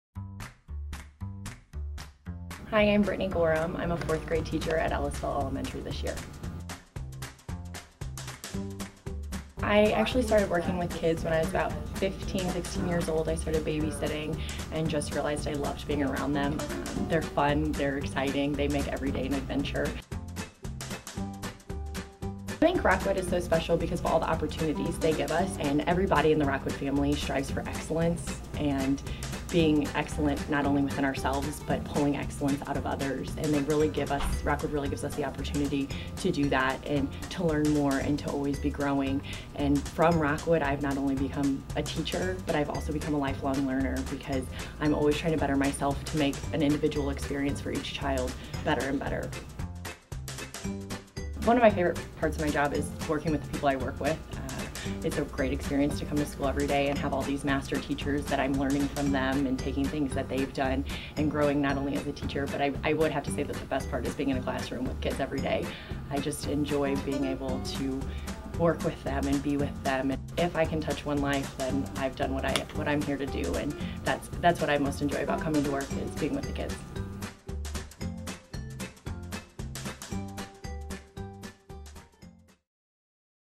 We are Rockwood with Brittany Gorham
Meet Brittany Gorham, a fourth grade teacher at Ellisville Elementary. Watch her video to learn more about her and hear about why she loves coming to work in Rockwood.